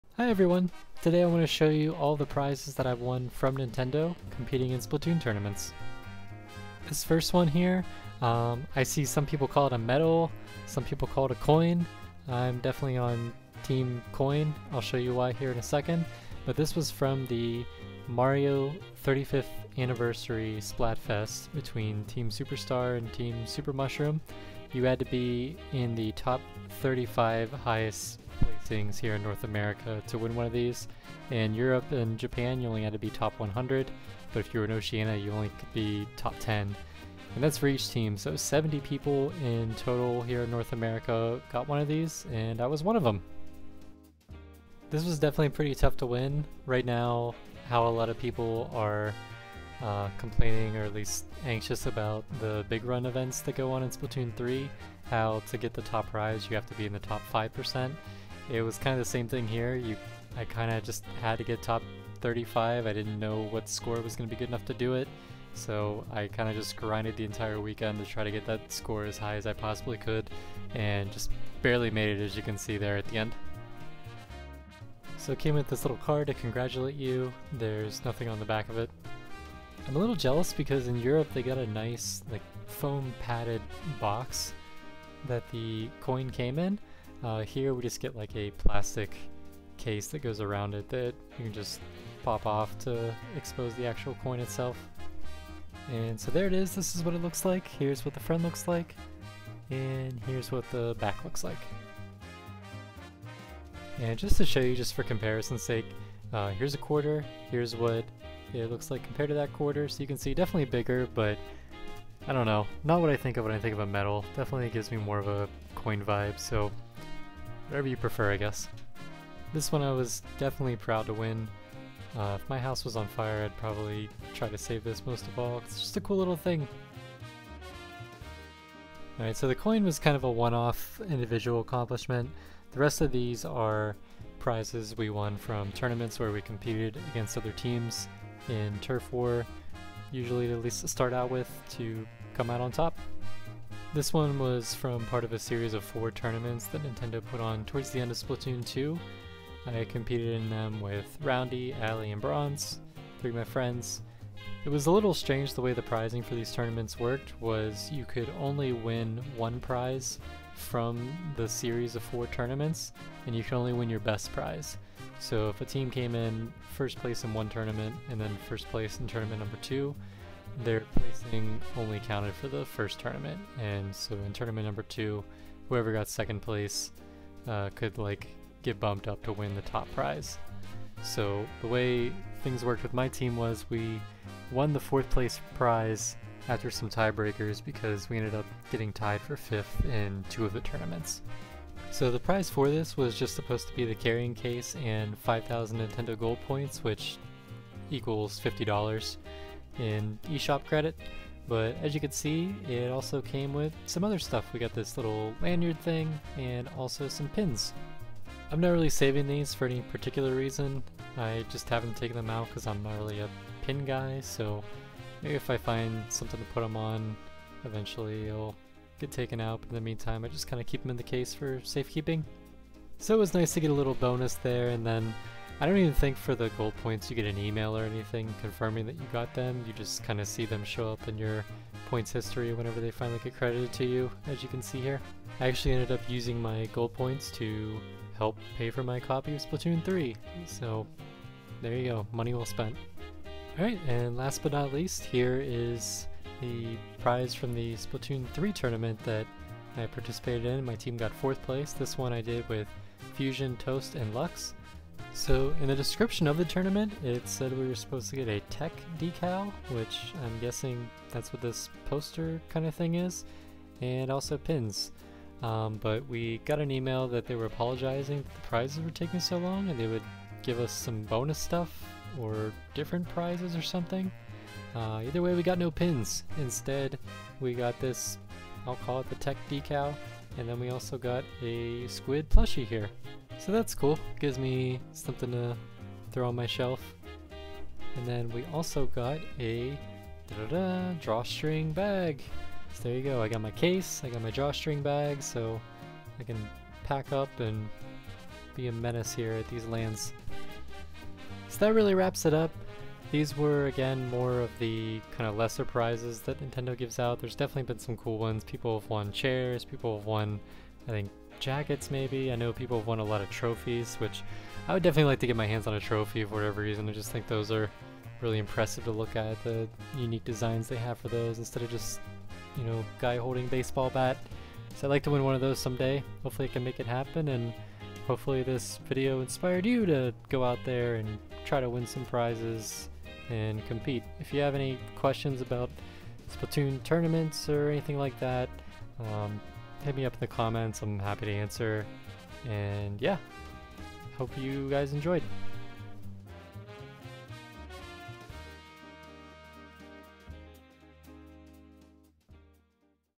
Every Prize I've Won from Nintendo for Splatting Squids
Just a fun little video showing you all the prizes that I've won for playing Splatoon 2 and Splatoon 3 in various events. While I haven't won the top prize in any of their tournaments, I have managed to get some pretty neat stuff!

I want to give a huge thank you to my teammates I played these tournaments with. I could not have done it without you all.
0:00 Splatfest Coin
2:08 Case and Pins
4:03 Tech Decal? and more!

If you'd like to watch me play Splatoon 3 live, be sure to check out my Twitch